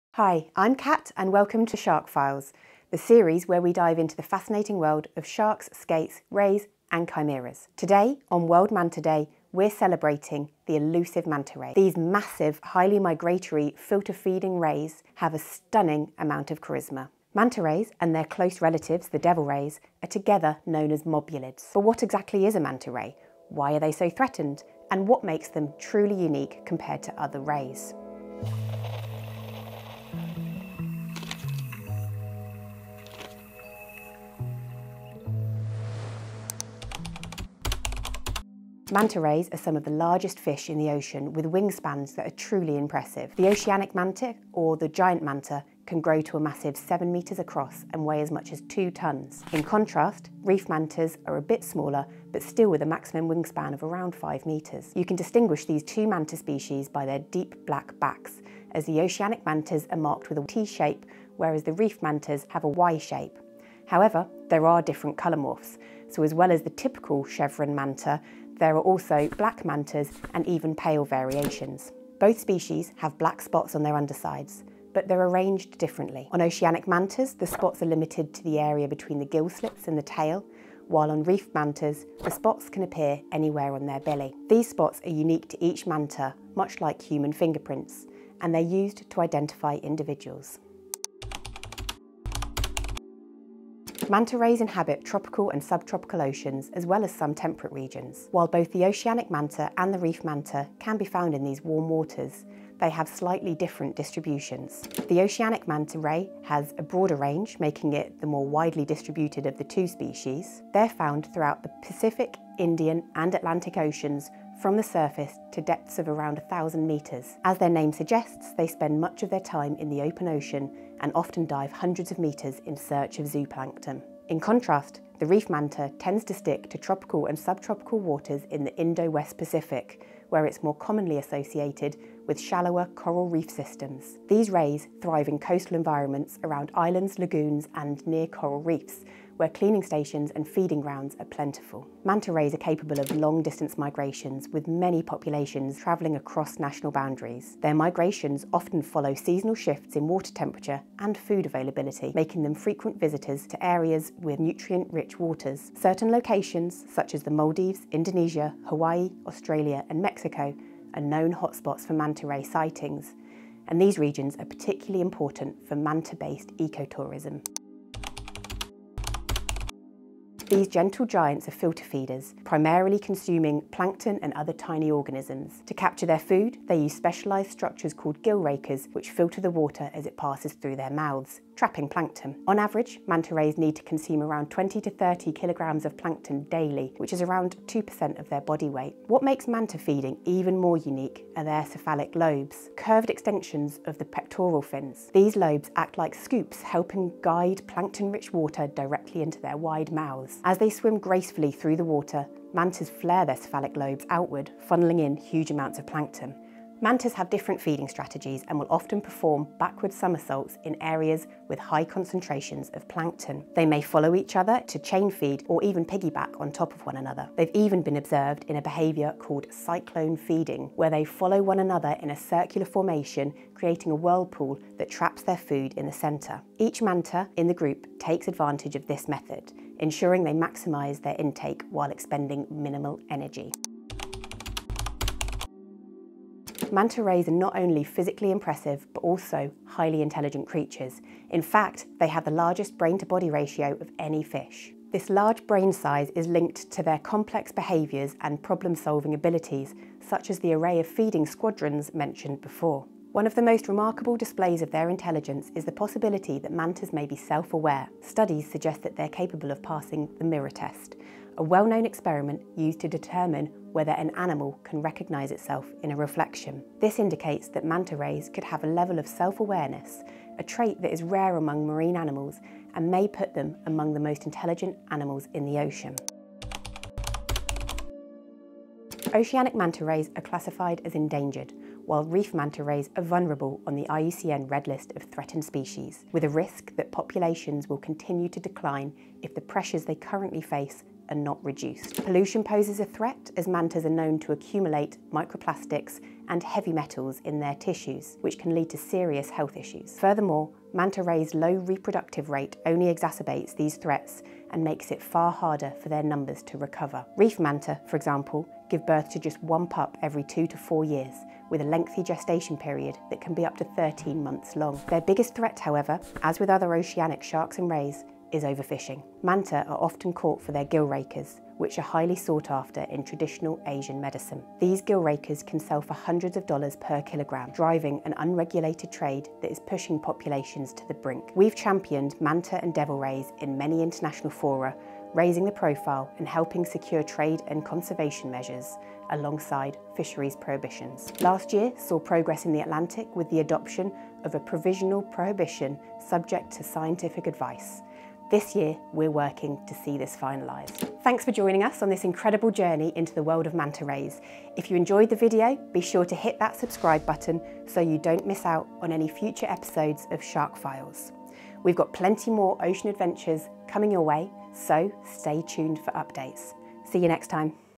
Our Ocean's Most Intelligent Fish? The Manta Ray
In celebration of World Manta Day (17th of September), this episode of Shark Files will focus on the Manta Ray.

Manta Rays are threatened by overfishing. Add your voice to the call for stronger fisheries management and conservation by signing the Big Shark Pledge at: